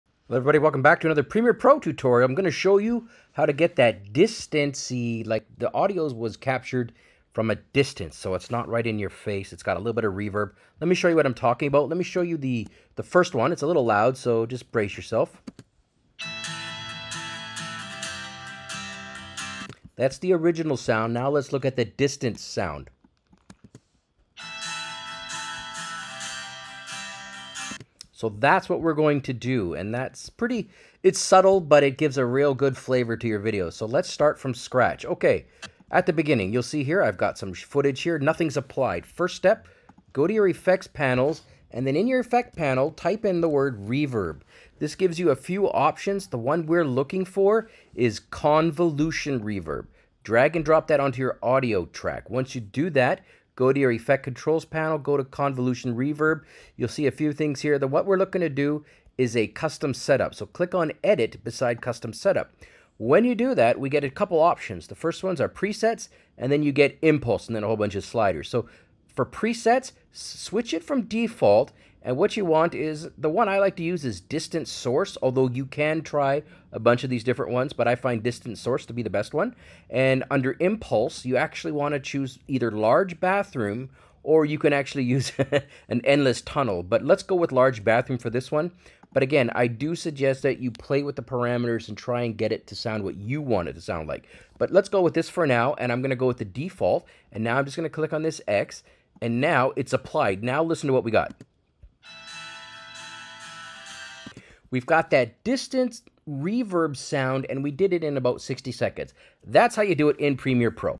How To Create A Distant / Large Room Audio Effect - Premiere Pro 2023
In this quick tutorial I show you how to create a distant audio effect, or a large room audio effect, using Adobe Premiere Pro 2023.  The first step is to load your audio onto the timeline.  Next, go to the effects panel and select and apply the convolution reverb effect effect.  Go to your effects control panel and click on custom setup - edit.  Change the preset to distant source and the impulse to large bathroom.  Adjust sliders as you see fit.